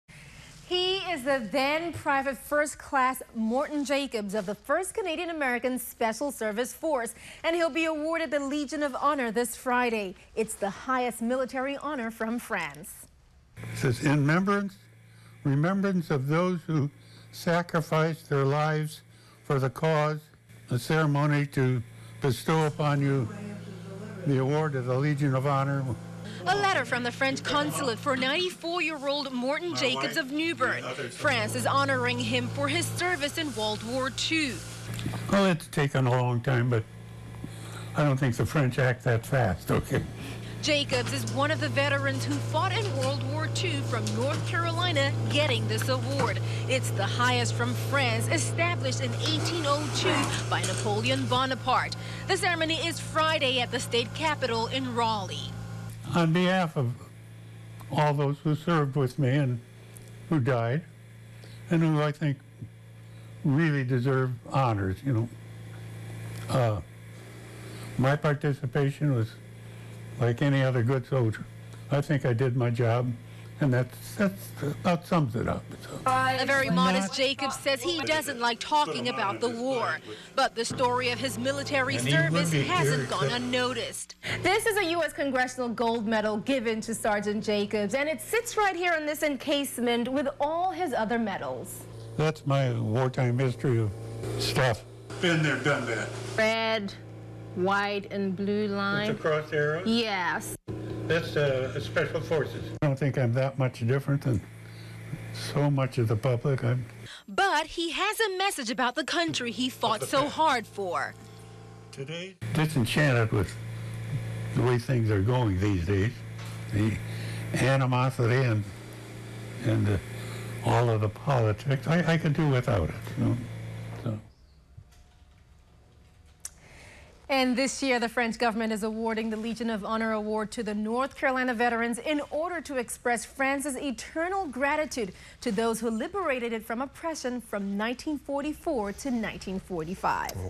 WW II VET GETS LEGION OF HONOR AWARD FROM FRANCE
Morton Jacobs of New Bern was a private first class for the first Canadian-American special service forces back in World War II. The government of France awarded him the Legion of Honor. Adaure Achumba reports